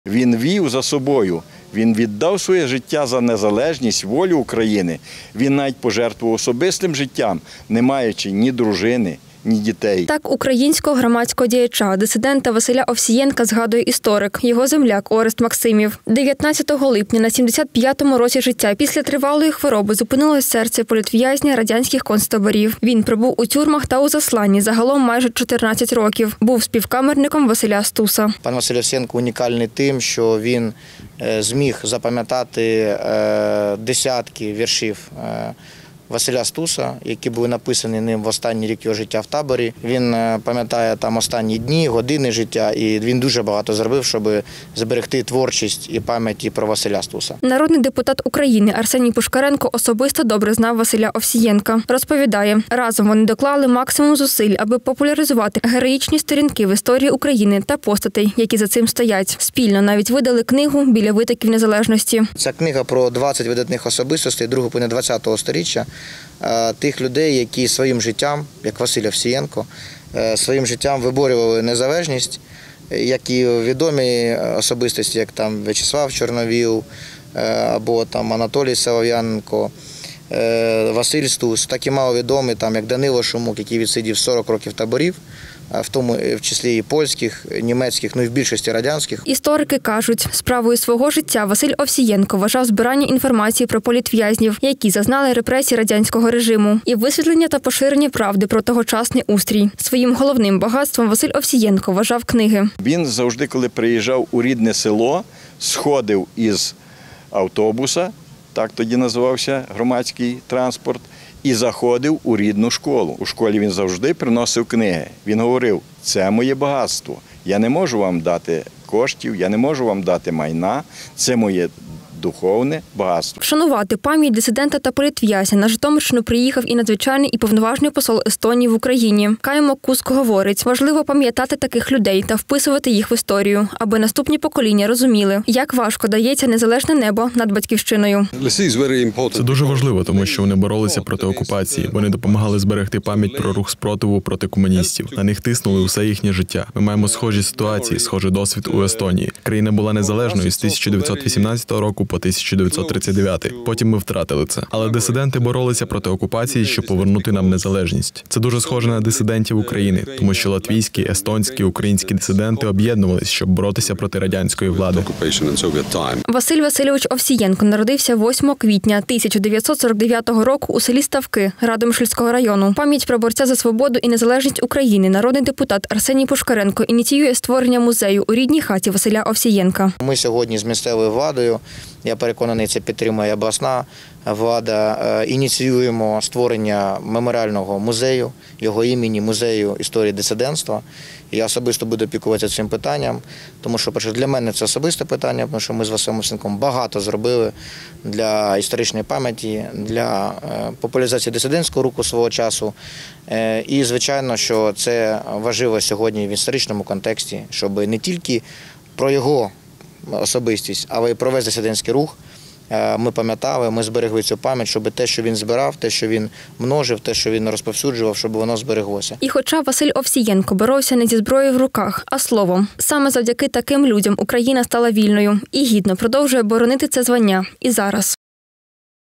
Пам’яті Василя Овсієнка: нардеп ініціює створення музею у рідній хаті дисидента
19-го липня пішов із життя український дисидент, громадський діяч, політв’язень радянських концтаборів Василь Овсієнко. Усе своє життя він присвятив висвітленню правди про радянський режим та збереженню історичної правди про дисидентський рух. В пам’ять про борця за Україну на Житомирщині, звідки родом Василь Овсієнко, ініціюють створення меморіального музею. Деталі  — у матеріалі